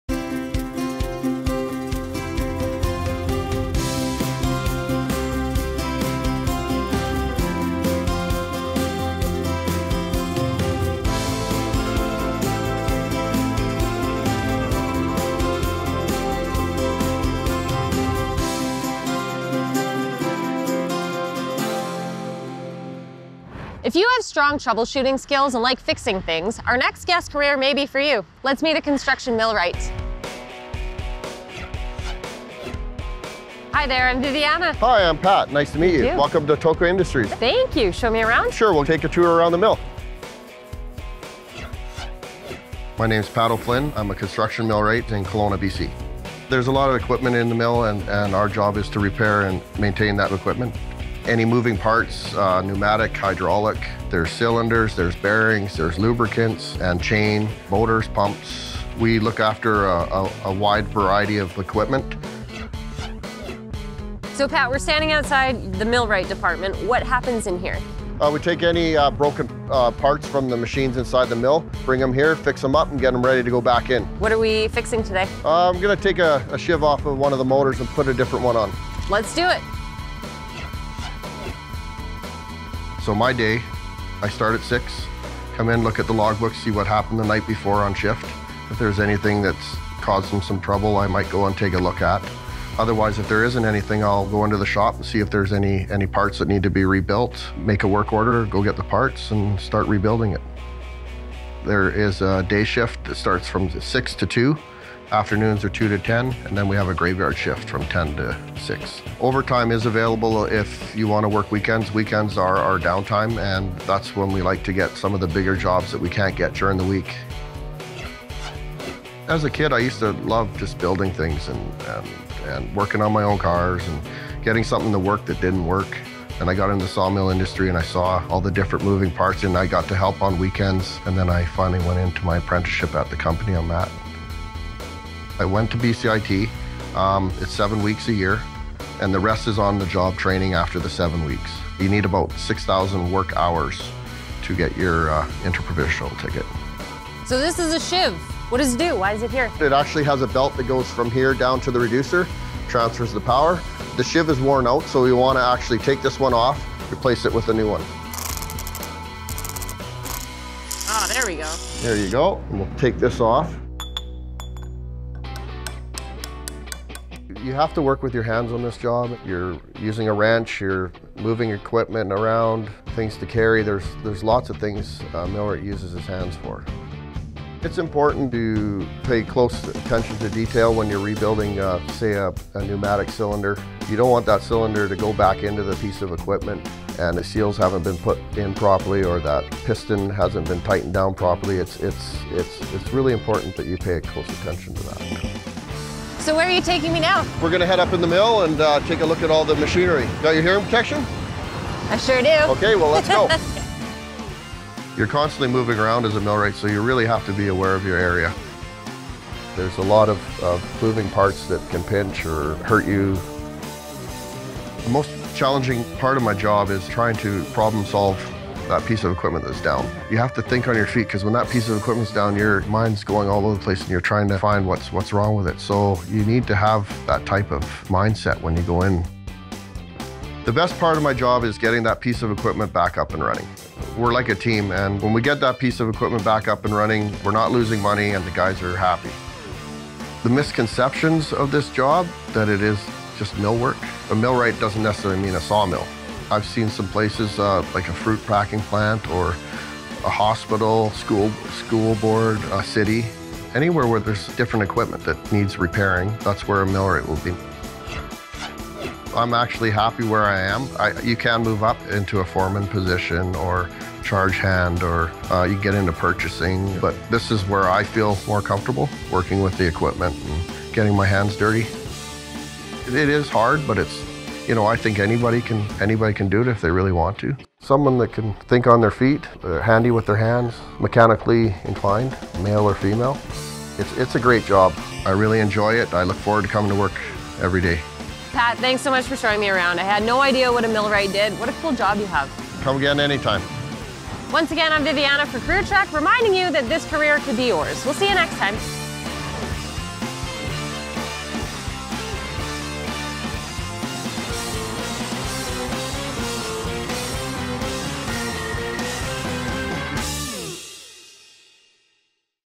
Construction Millwright (Episode 105)
Viviana meets with the person who maintains and repairs equipment in a Kelowna sawmill. For this construction millwright, the best part of his detail-oriented career is “getting that piece of equipment back up and running.”